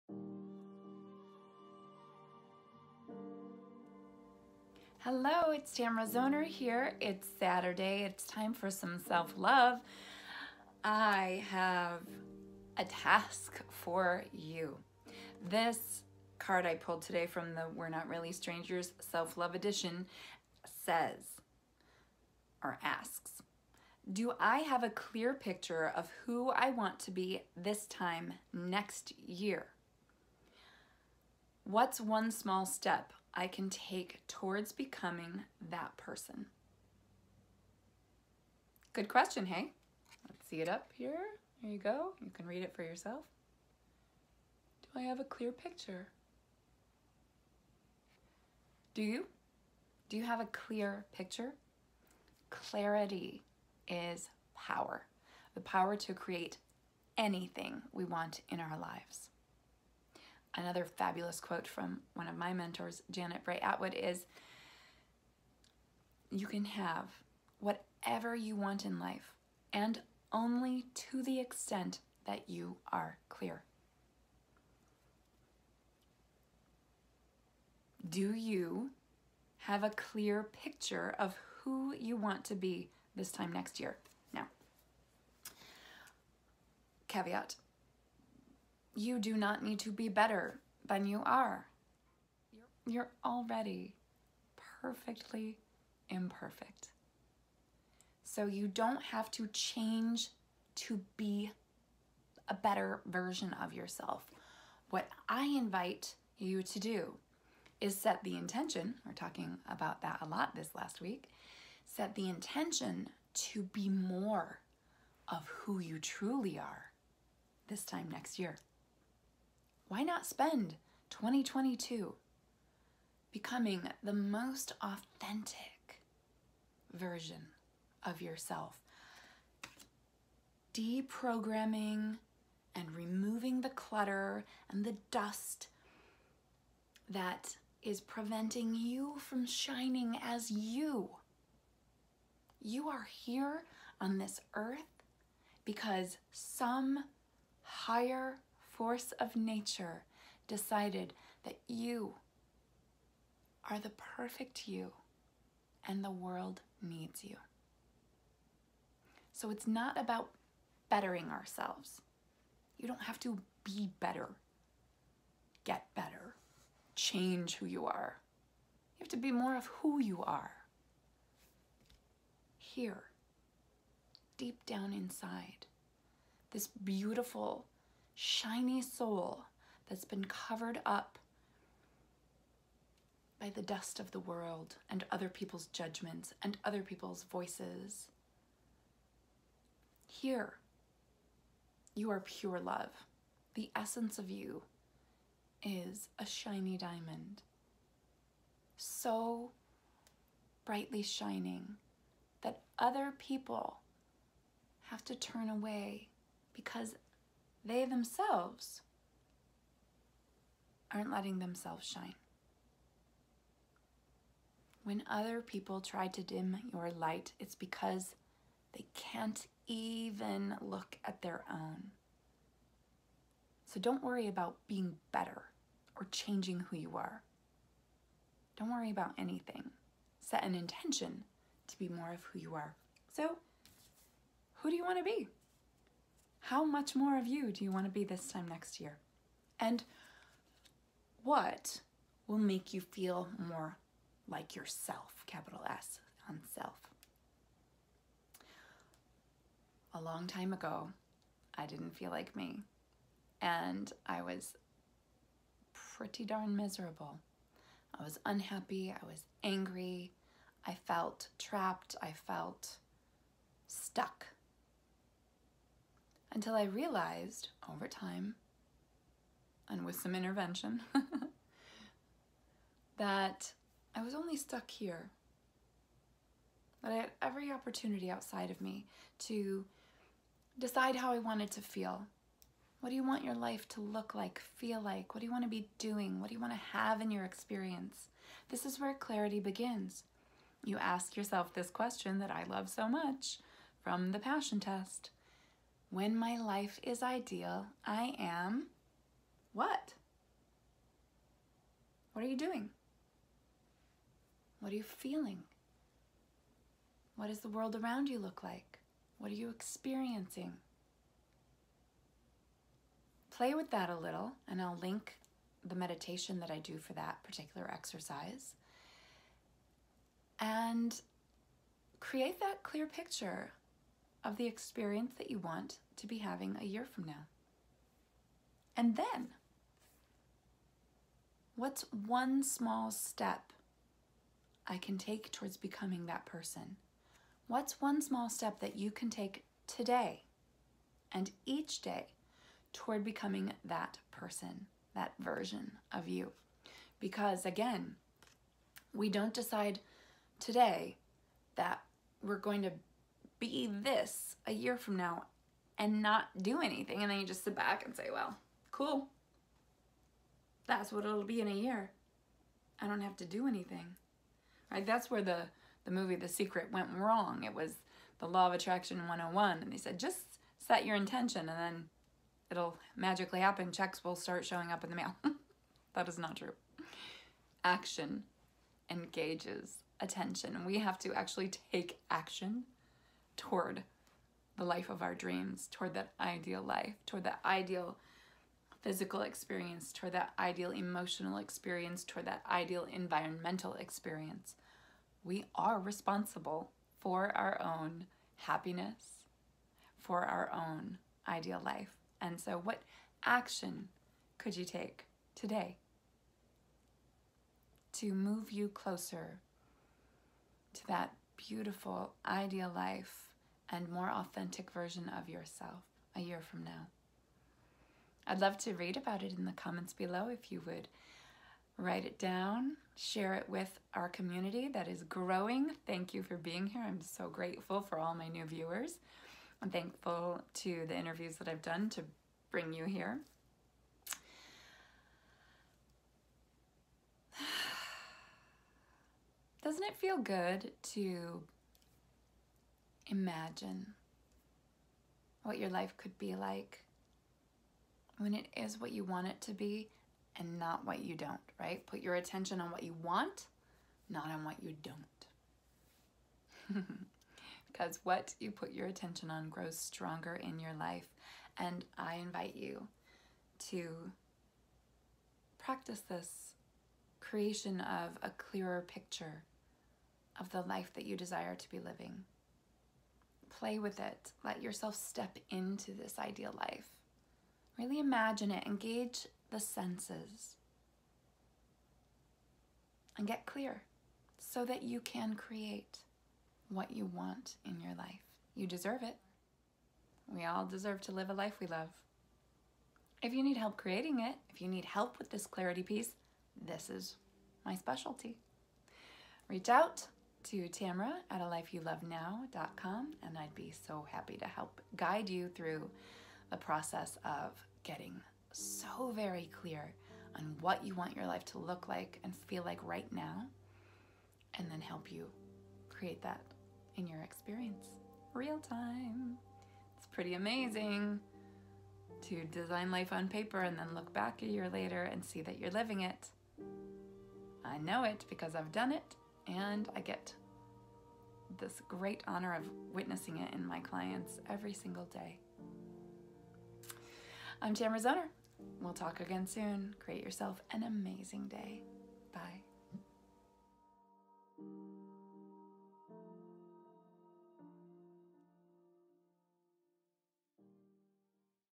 Self Love Saturday - A Year from Today
Join me for our Saturday Series all about Self Love! We'll talk about it, break it down, and practice it each week so that you can create a beautiful, loving, and compassionate relationship with yourself.

Ready to go deeper? to work with me 1:1 and take The Passion Test with me!